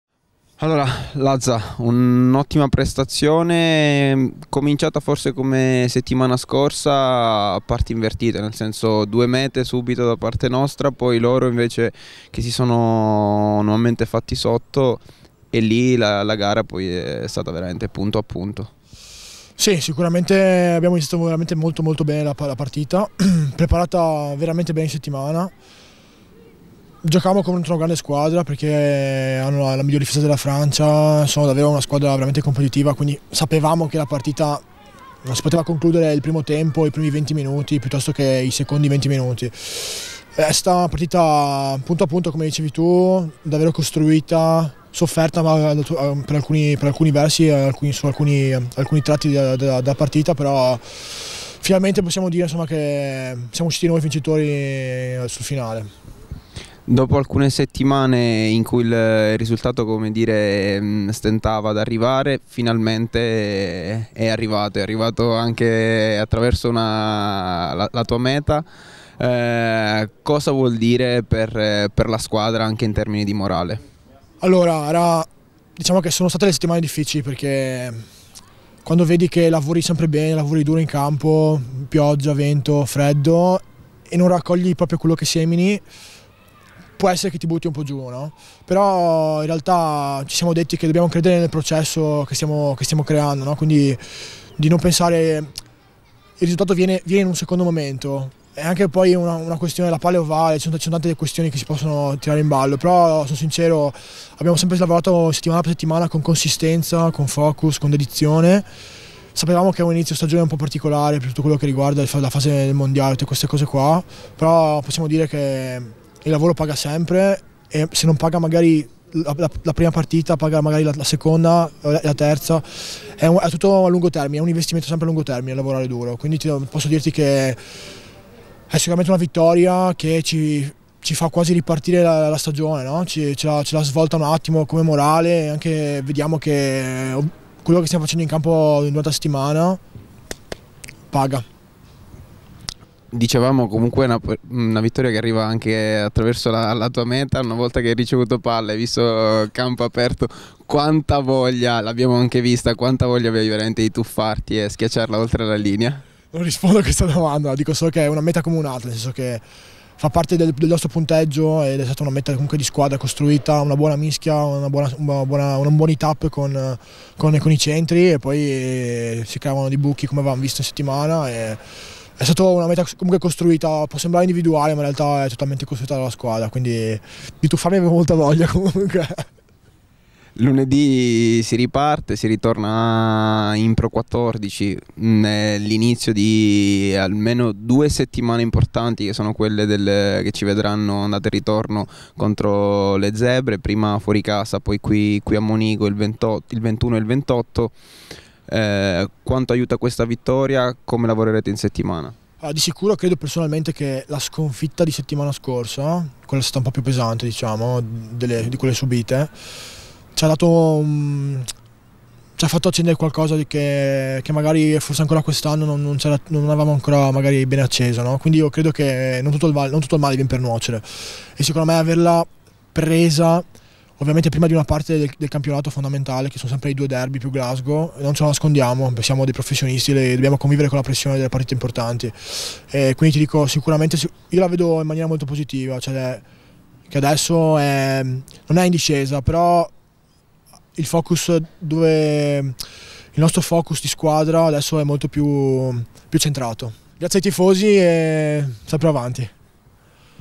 Lazzaroni: "Una vittoria che dà morale in vista delle prossime fondamentali gare"
Marco Lazzaroni, ai microfoni di BEN TV, analizza la vittoria contro Lione